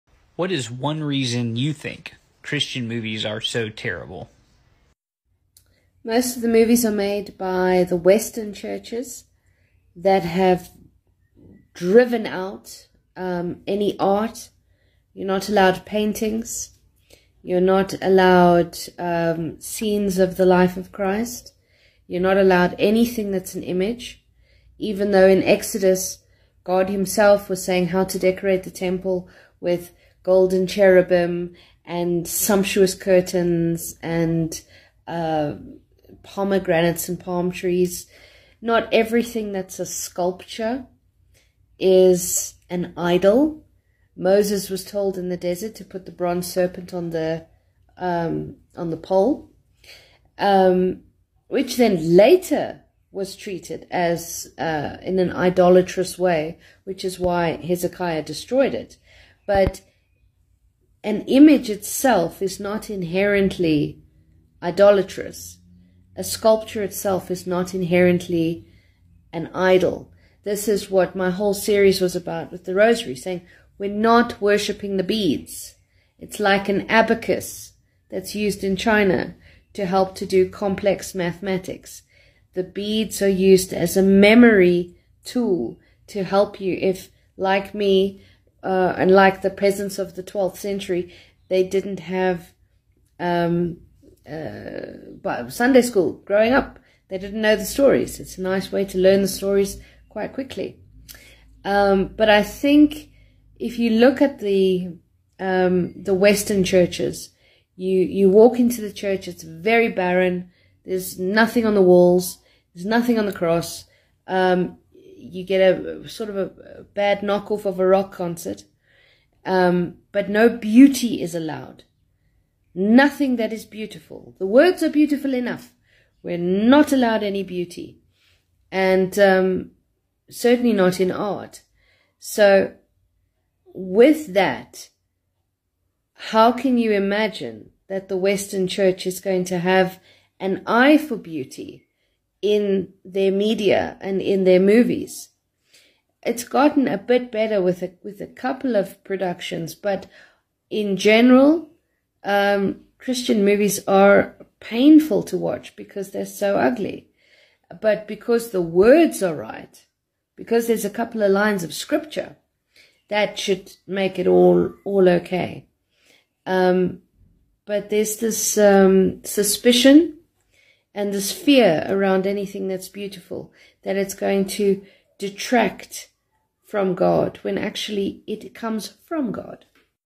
Why are Christian movies so terrible?
A discussion about Christian film and a possible reason for the poor quality.